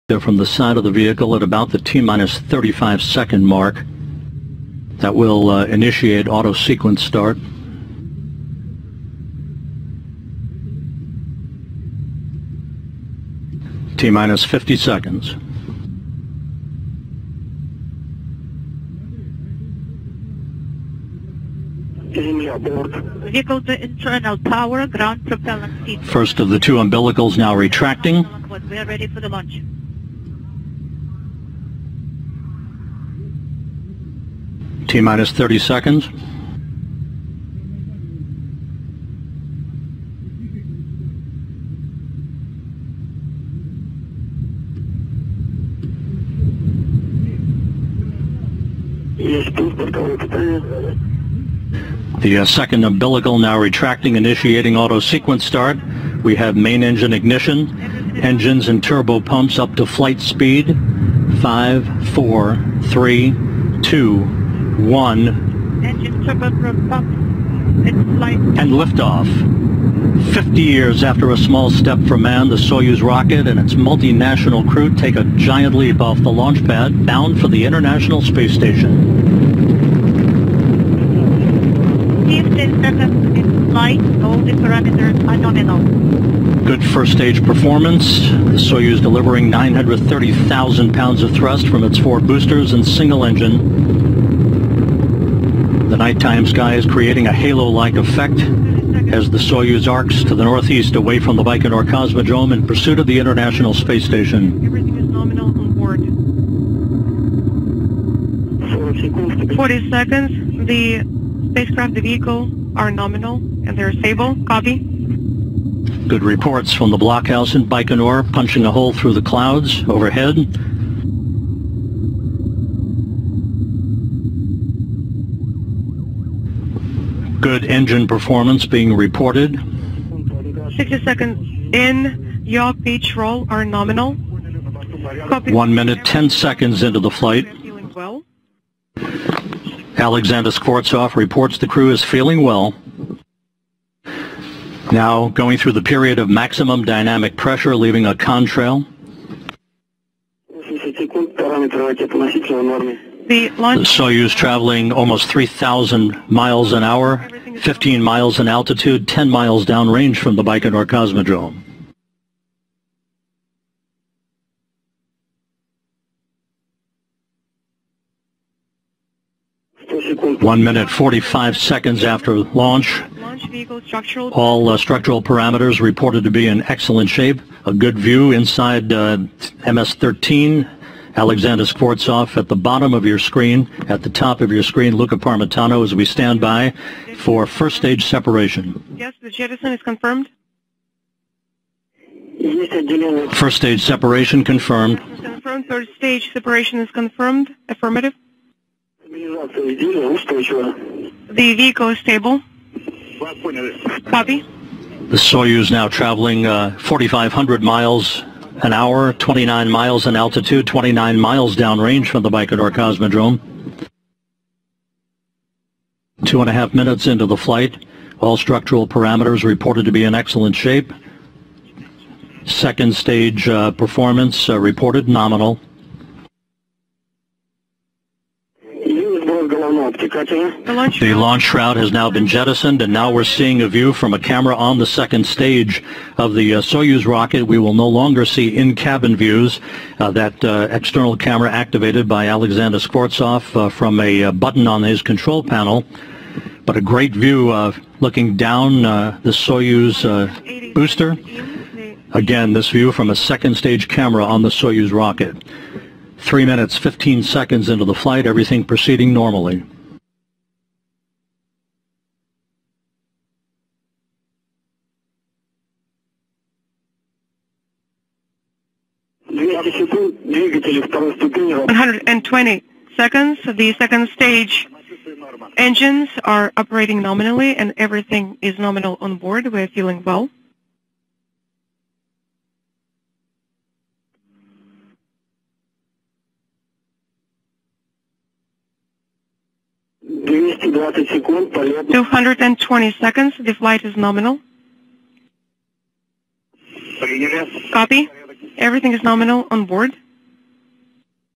Astronauts launched to ISS in Soyuz from Baikonur
Video courtesy of NASA
Nominal Orbital insertion of Soyuz